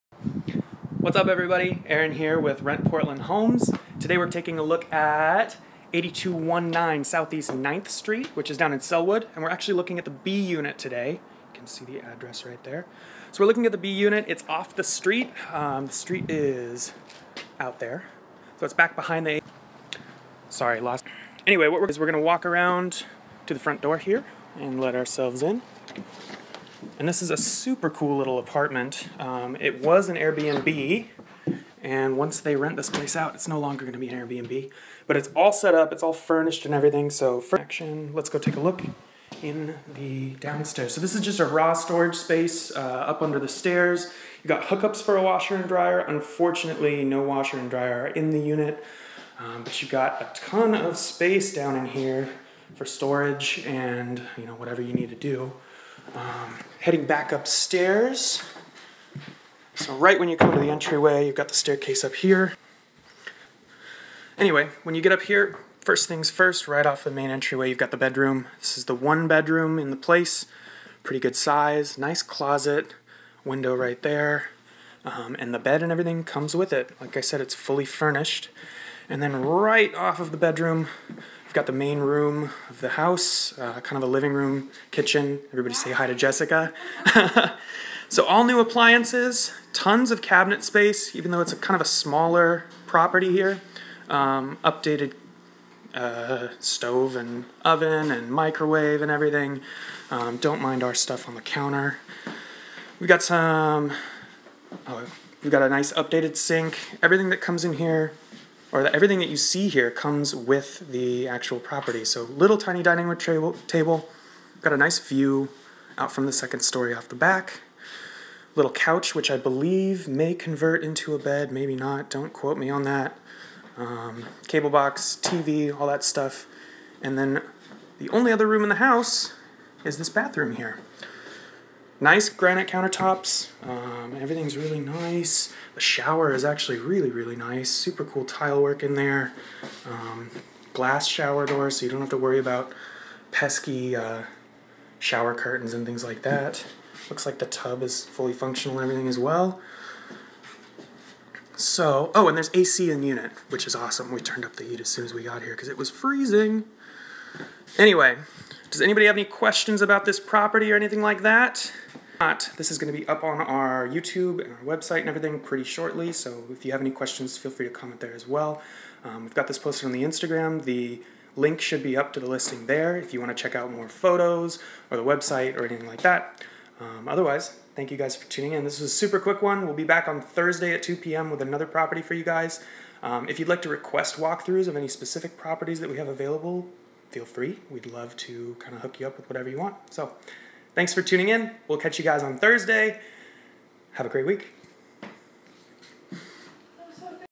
Live Walkthrough: 8219 SE 9th Ave - Unit B
Aaron takes us through this beautiful fully furnished condo in Sellwood live on our Instagram story. Tune in Tuesdays and Thursdays at 2pm to catch more walkthroughs like this one!

“Enjoy the Sellwood lifestyle in this upgraded fully furnished one bedroom. May be rented unfurnished if needed. Perfect location near everything, bike or walk to restaurants, parks and stores!! Two blocks from New Seasons and 13th Avenue that is jam packed with coffee shops, bars, restaurants and fun shops to explore. Only four blocks to the Sellwood Bridge that connects you to John's Landing, the South Waterfront, OHSU and Downtown Portland!

Newly built in 2016 this one bedroom unit is beautiful with many upgrades. The living space is located upstairs. Downstairs off the entryway and stairs is the utility room which includes washer dryer hookups. Living room and kitchen are open and feature West facing windows perfect for evening light. Stainless steel appliances are included in the kitchen. Air conditioning is included for your convenience. Beautifully appointed bathroom includes tub with glass shower doors.”